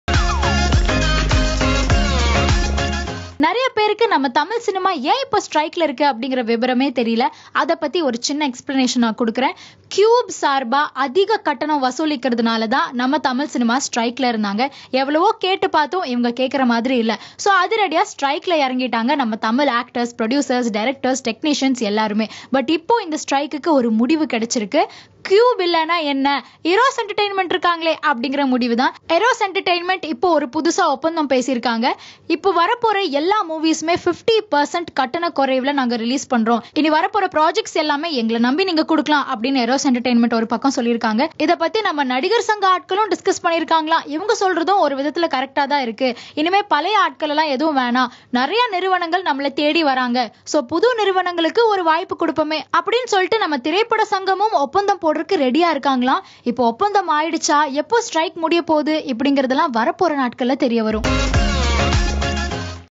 TFPC joins hands with another Digital Service Provider | First Frame
The Tamil Nadu Film Producer's Council (TFPC) got into an agreement with a new Digital Service Provider named AEROX